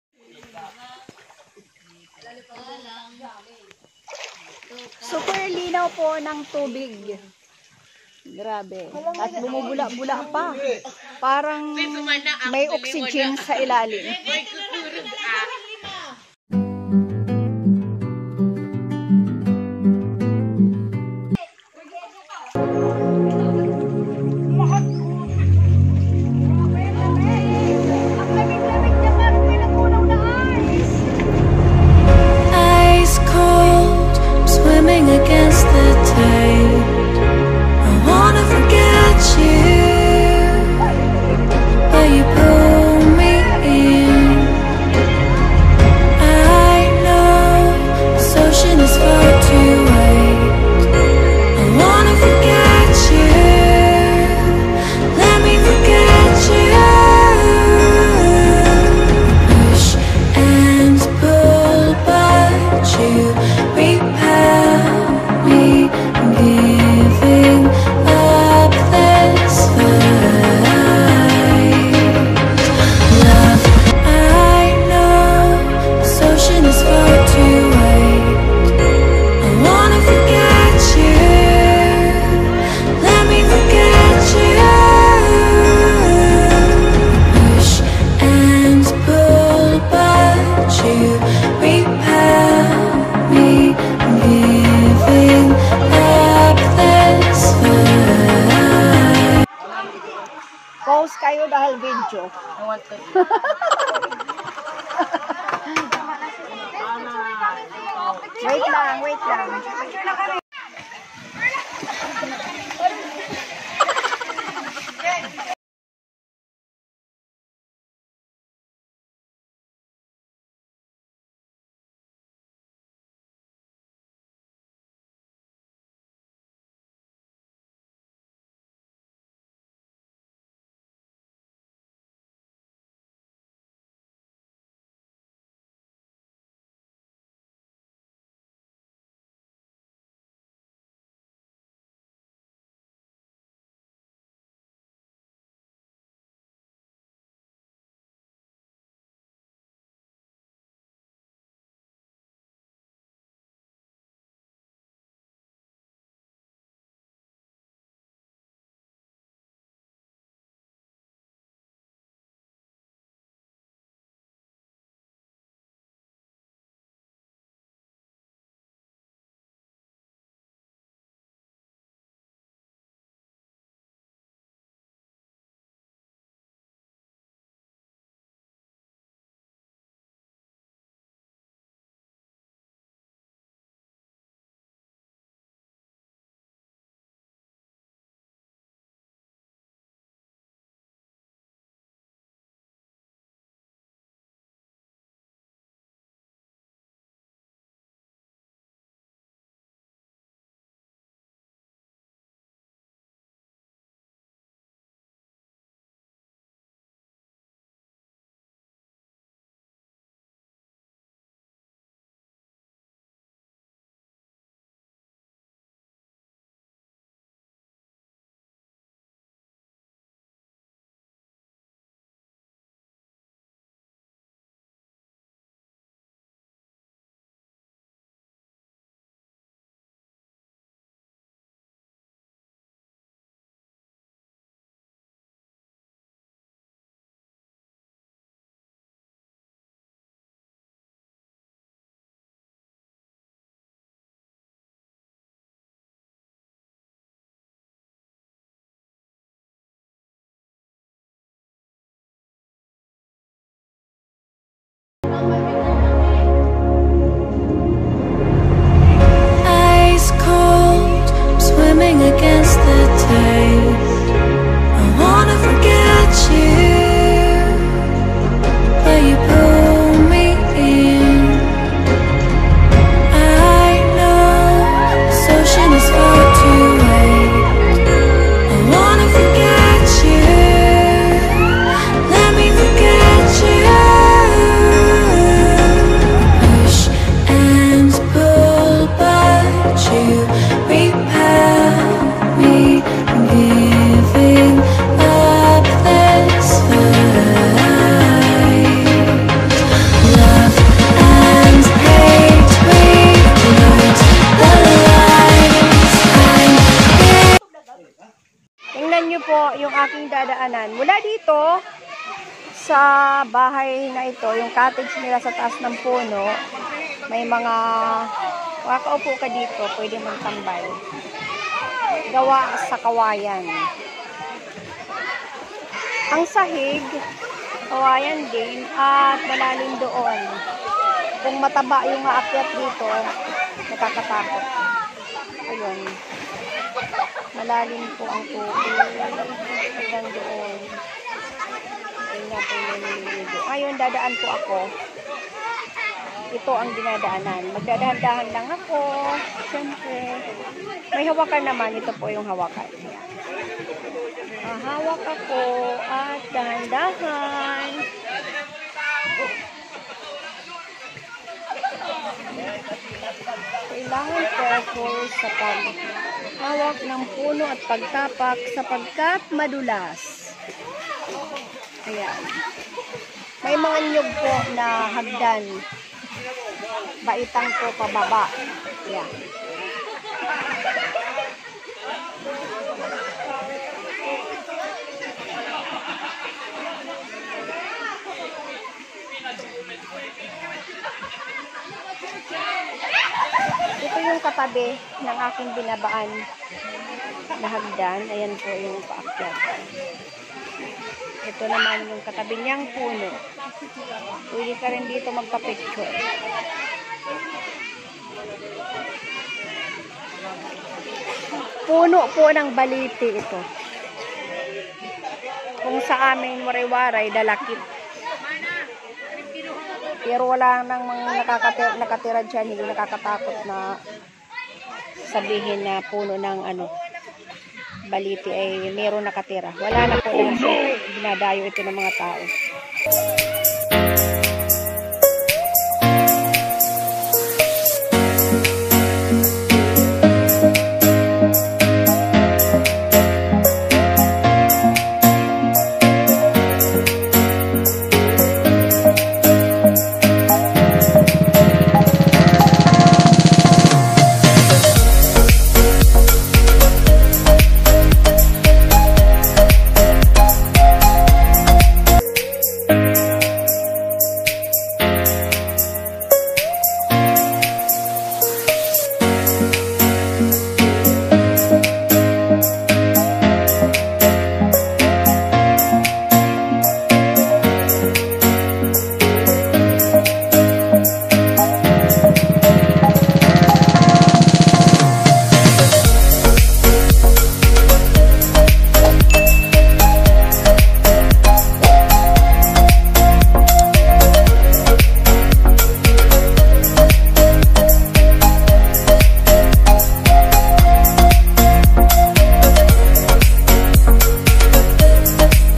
MALIHI-AW COLD SPRING#CHOIR TOUR(Part 2)
Experience the hidden Cold spring in Malinao Taft Eastern Samar.